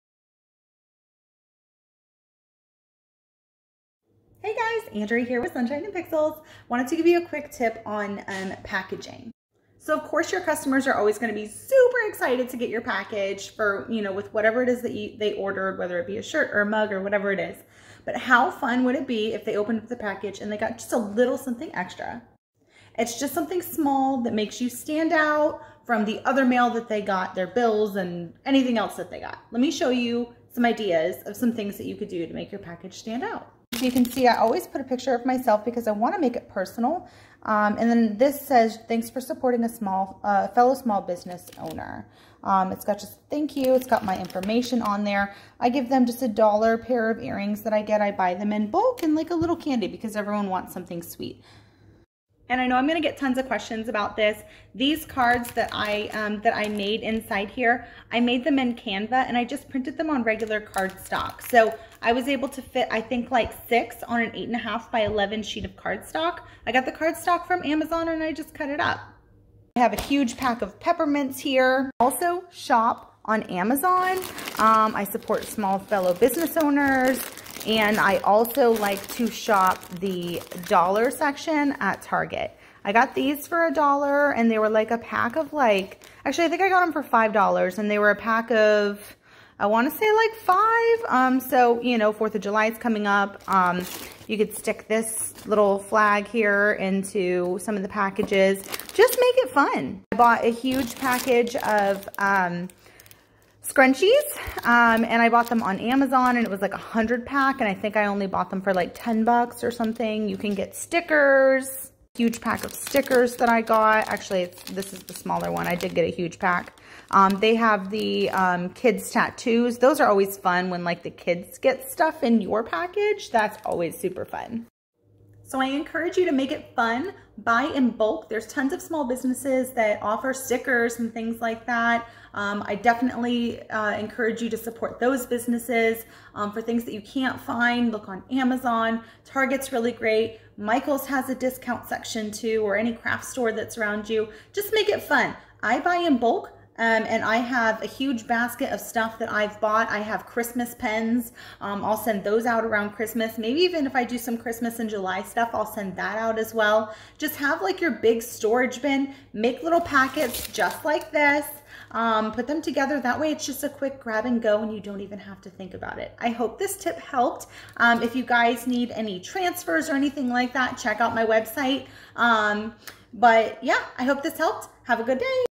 Package your orders to STAND OUT
how to package your orders to STAND OUT without cutting into your profit.
All of the items listed here can be found in my Amazon store but you can also shop local, shop other small businesses, major craft stores clearance sections and more!! get creative. Your customers will aprpeciate the extra mile.
I hope this helpful! Please feel free to join our facebook group for weekly sales and live Q&A

AMAZON STORE

XoXo
Andrea